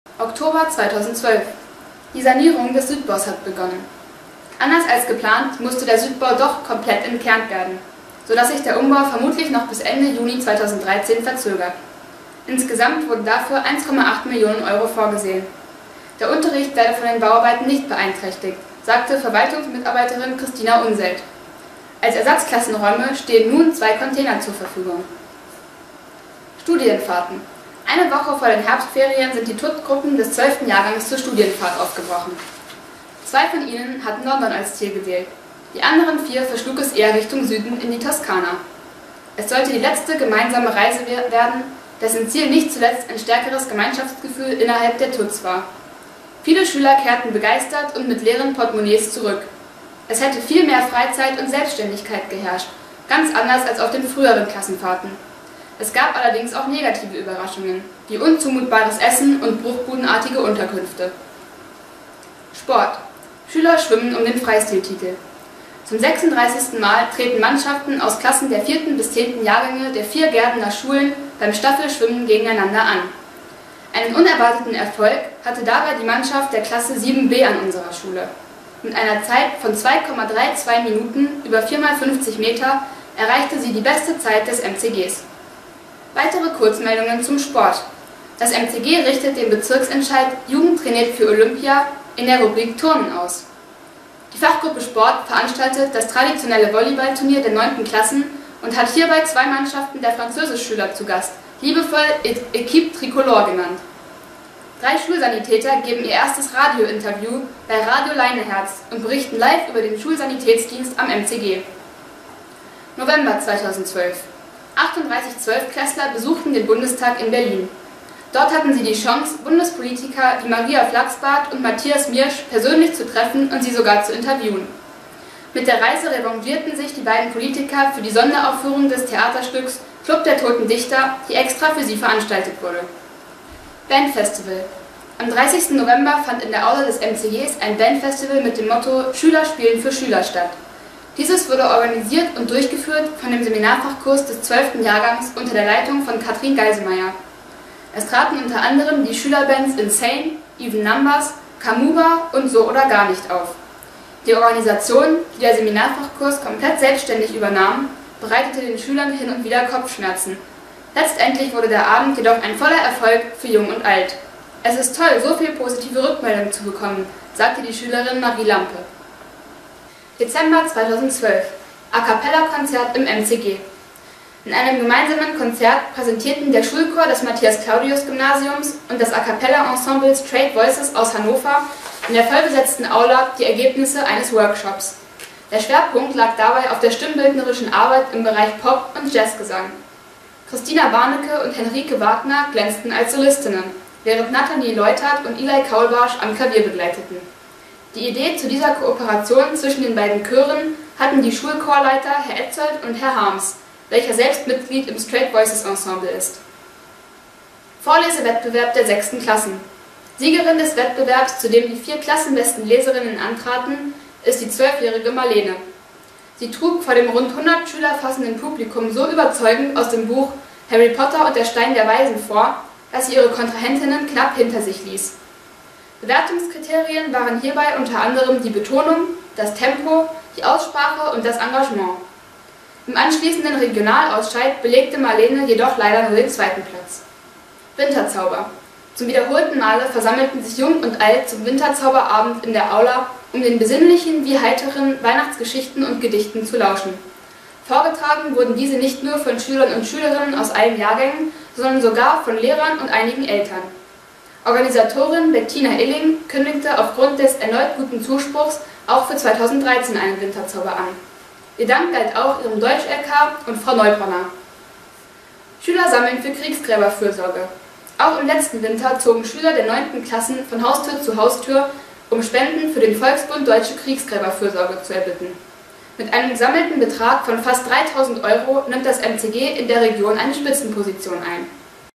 MCG-TV - Jahresrückblick 2012 Teil 3 - Theater am MCG
MCG-TV - visuelle Einblicke in das Schulleben

Jahresrückblick 2012 - Teil 2

Sanierung des Südbaus
Studienfahrt 2012
Schwimmwettbewerb
Neues aus der Fachgruppe Sport
Schulsaniätsdienst
Besuch des Bundestages
Bandfestival
Acapella-Konzert
Vorlesewettbewerb
Winterzauber
Kriegsgräberfürsorge